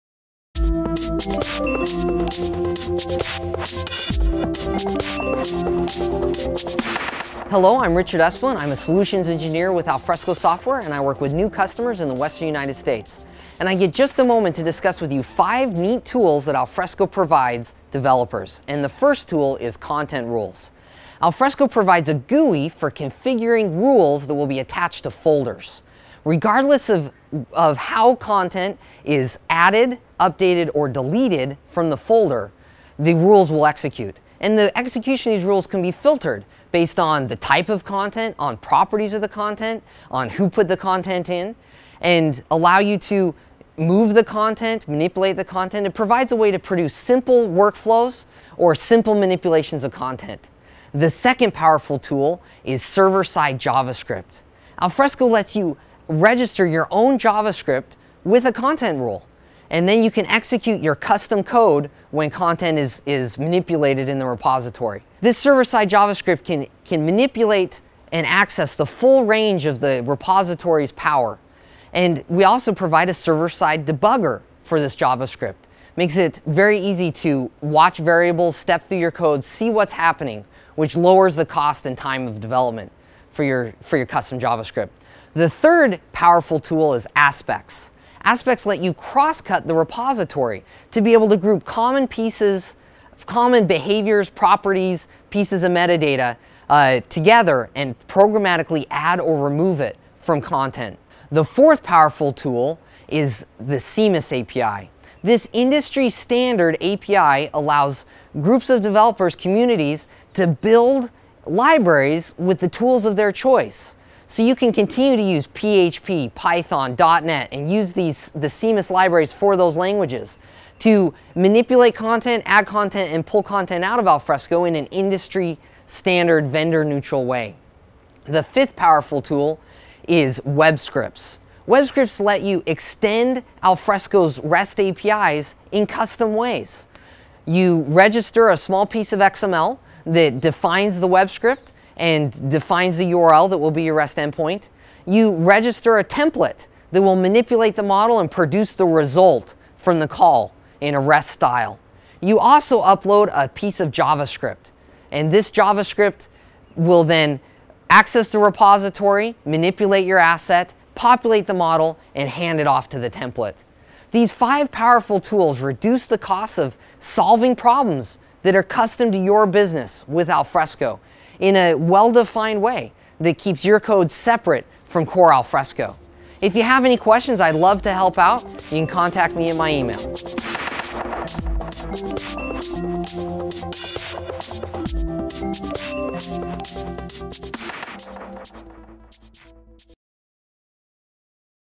Alfresco QuickTake: 5 Tools Alfresco Provides Developers
Richard Esplin of Alfresco describes 5 Tools that Alfresco provides to developers.